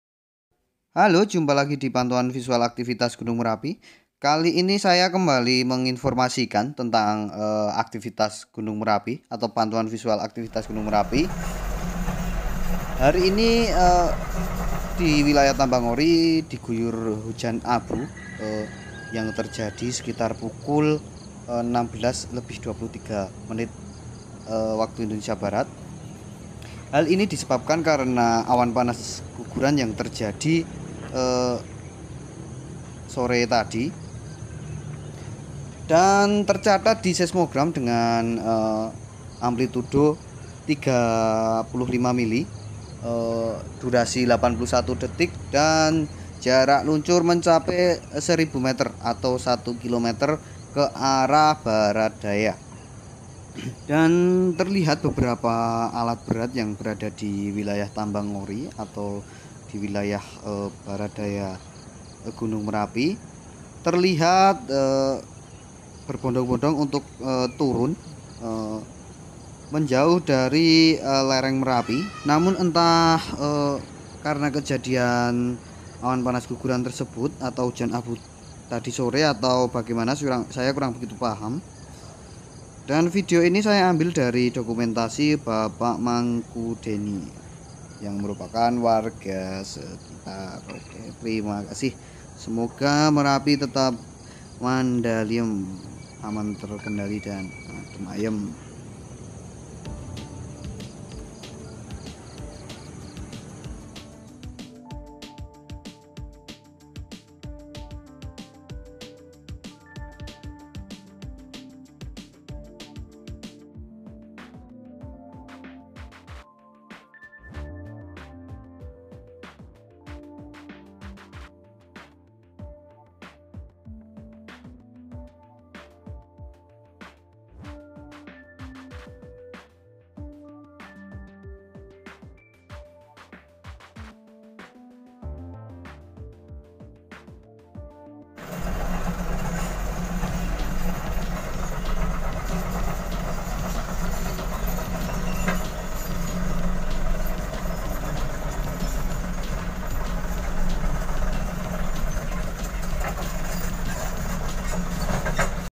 Awan panas guguran merapi hingga hujan abu di barat daya merapi
Live merapi hari ini terjadi awan panas dengan jarak luncur 1000 m dengan amplitudo 35mm durasi 81 detik ke arah barat daya gunung merapi,awan panas ini megakibatkan hujan abu di wilayah ambang ngori